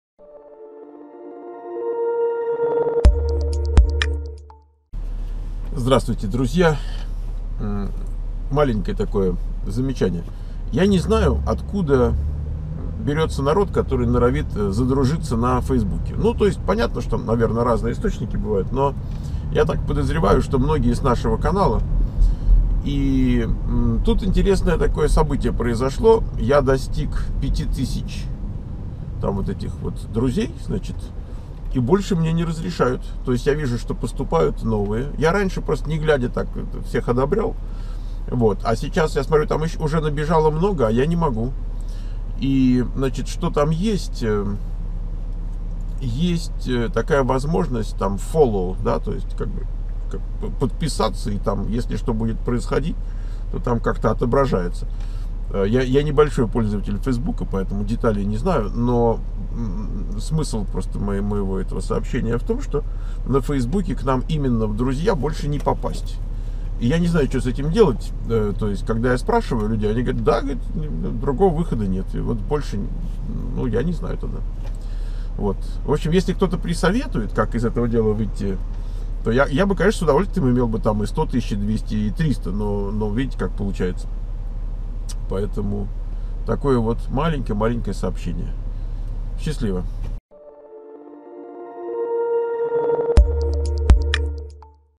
США 4099: Facebook - дошел до 5000, больше друзей не разрешают простым людям
Facebook - дошел до 5000, больше друзей не разрешают простым людям
Ссылки на канале SiliconValleyVoice /школа Михаил Портнов: